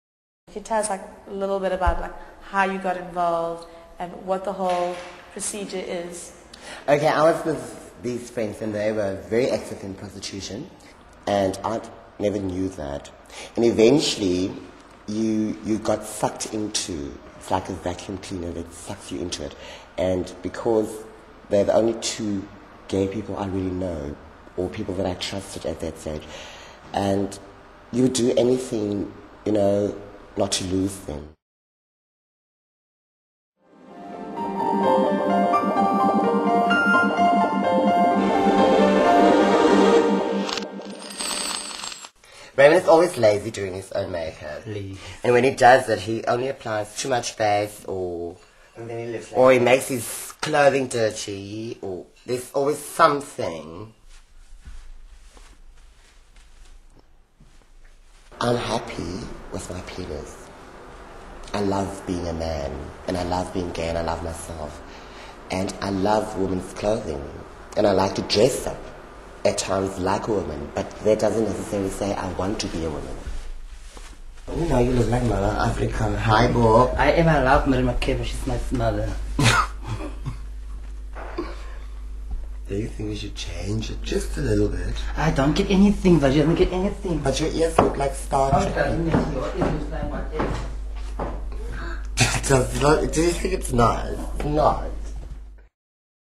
Unmasking Mavis Documentary
A short documentary that explores various sub-cultures in South Africa.